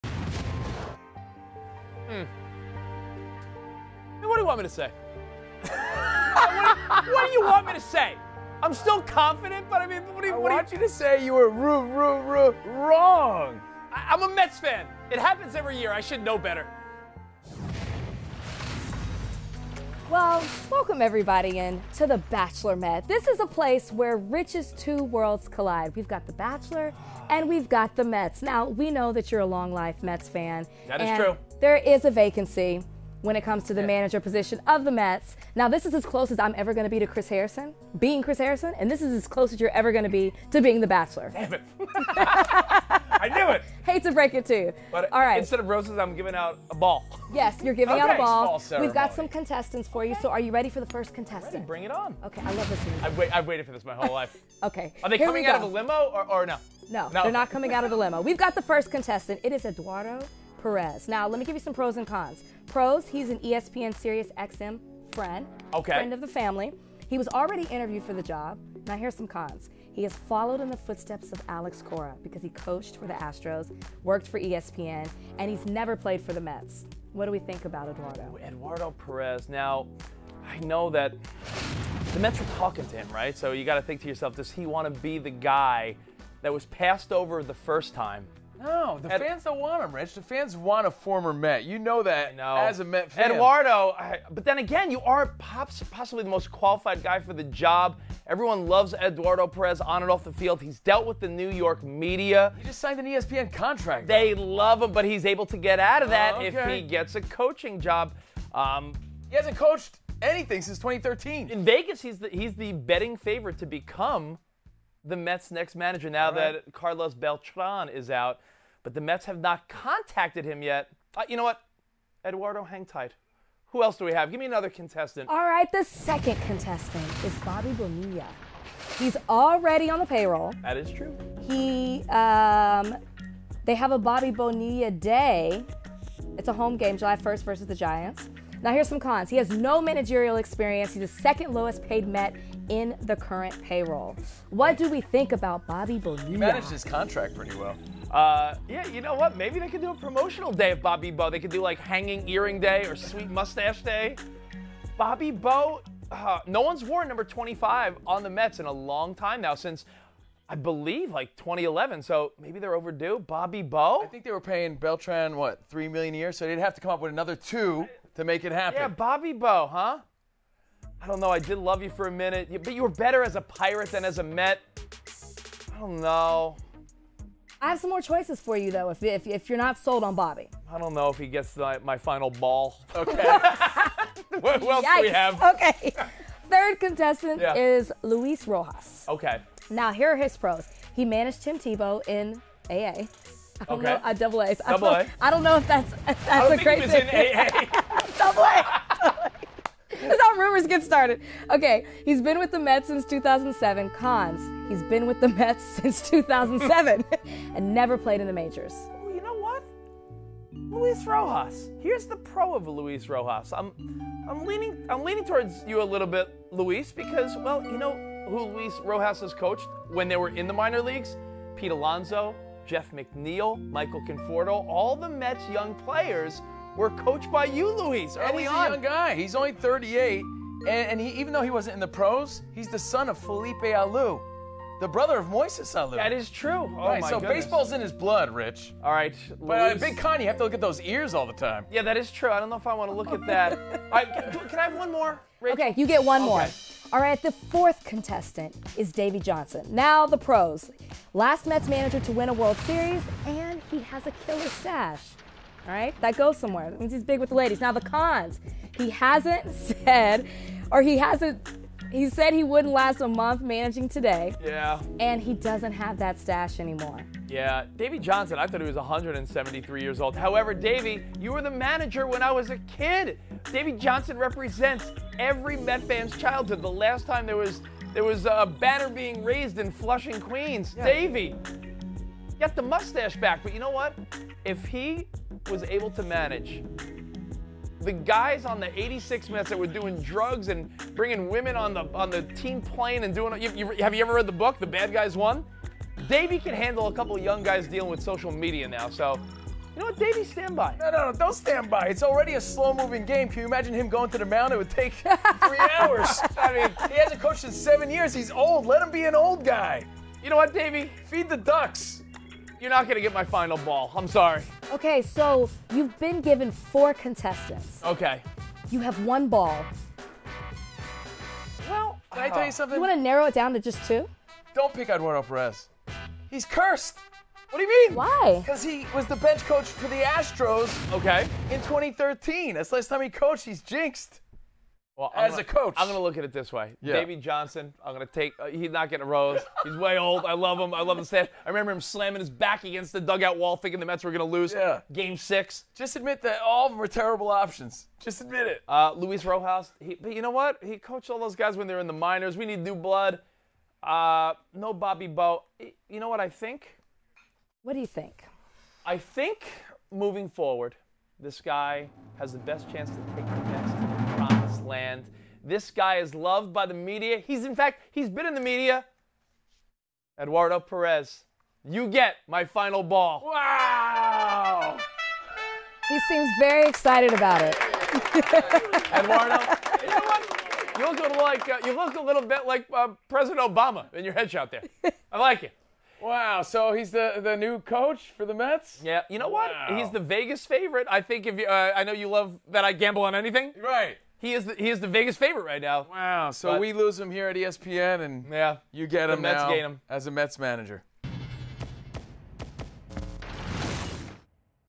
The BachelorMet: Rich Davis looks for the perfect Manager for the New York Mets
4 Bachelor-Mets are looking to win Rich Davis Fan boy heart.
He has to 4 eligible bachelors looking to score the coveted position as Manager of the New York Mets.
The 4 Bachelor Mets:
- Eduardo Perez
- Bobby Bonilla
- Luis Rojas
- Davey Johnson

Who will win Rich's Heart ???